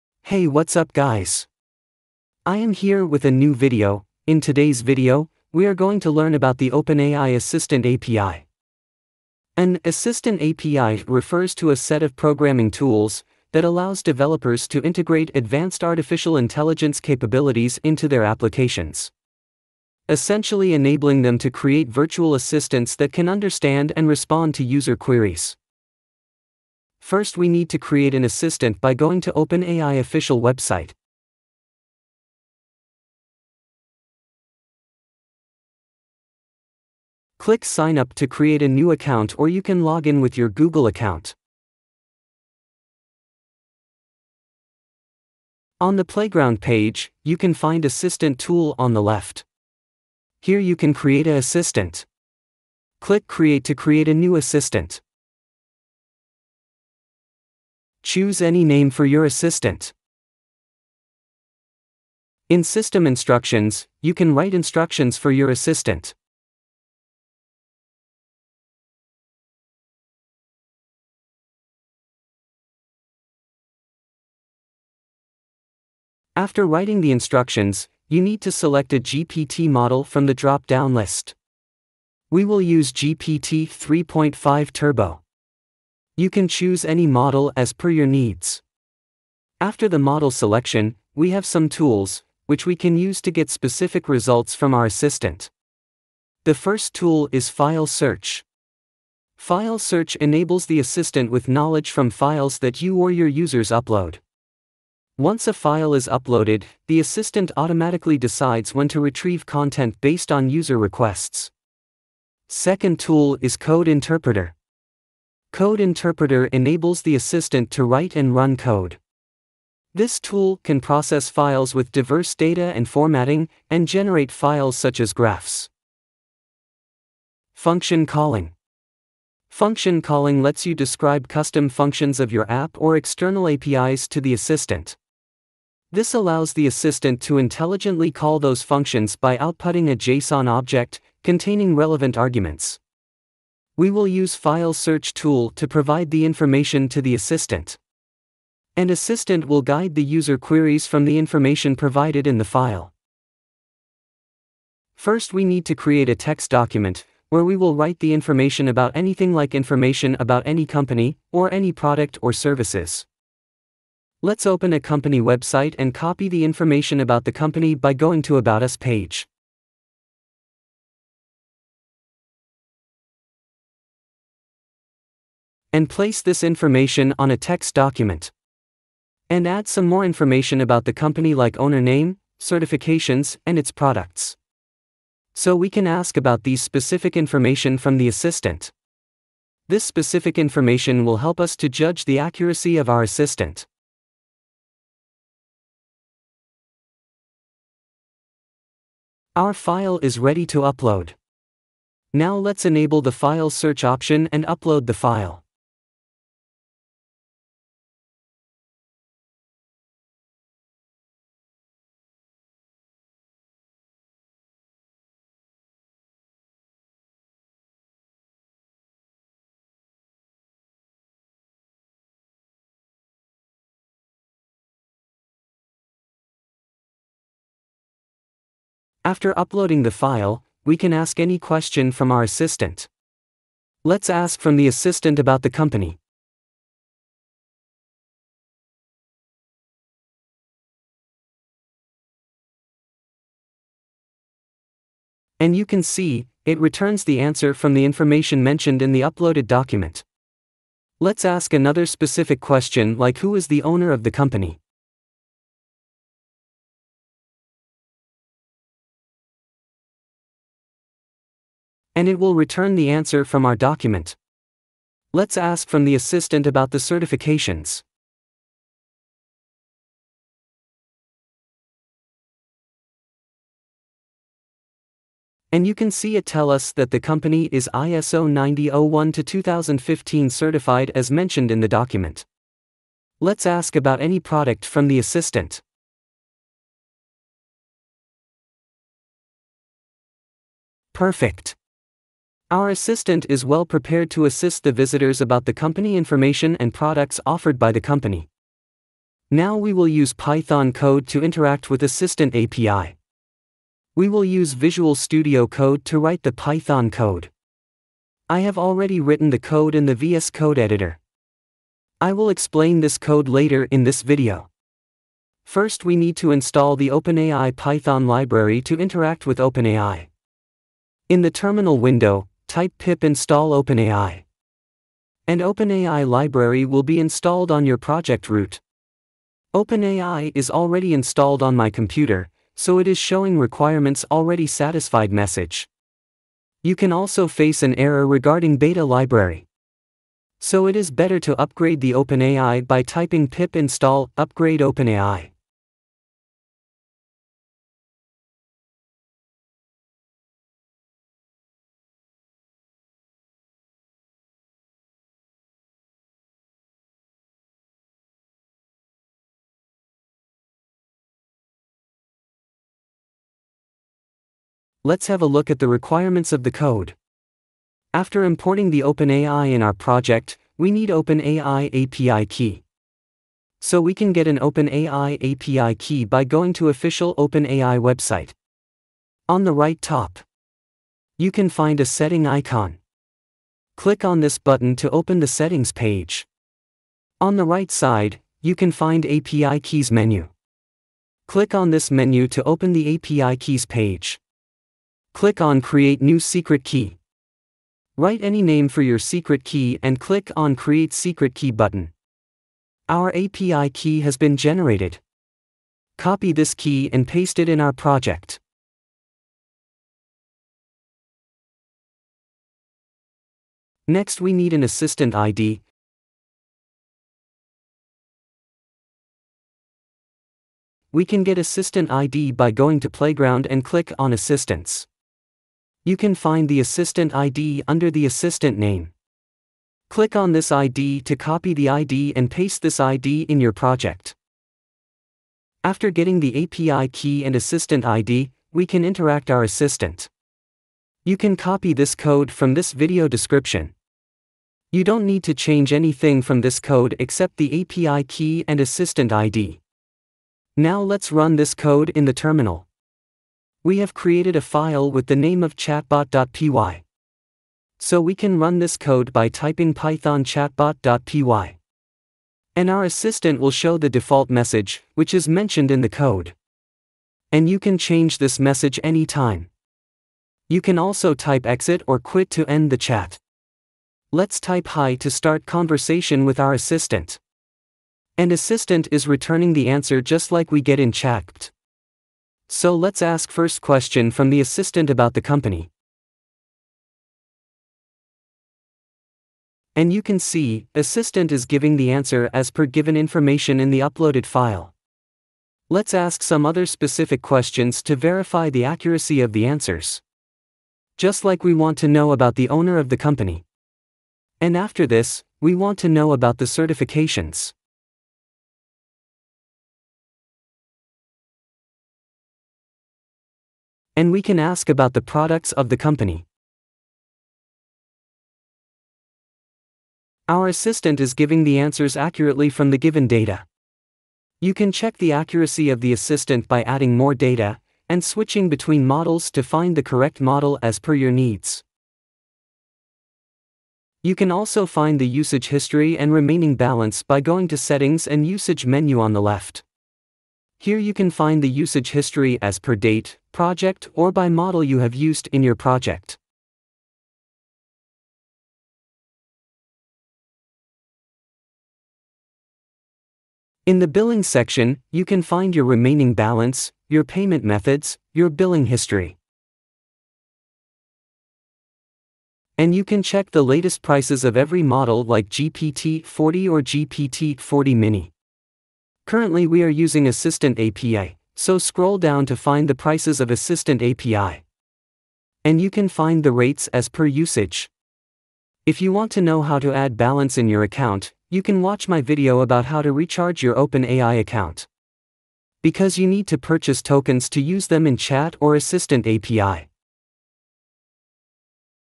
OpenAI Assistant API course for beginners || OpenAI assistant API Python || OpenAI Tutorial
Learn how to create and use OpenAI Assistant API in your Python projects! In this video, we'll explore what the OpenAI Assistant API is, its capabilities, and how to integrate it into your Python applications. From understanding the basics of natural language processing to building conversational AI models, we'll cover it all. We will cover How openAI Assistant's work, How can we build assistant in Playground. How to use Open AI assistant api with python code to interact with assistant.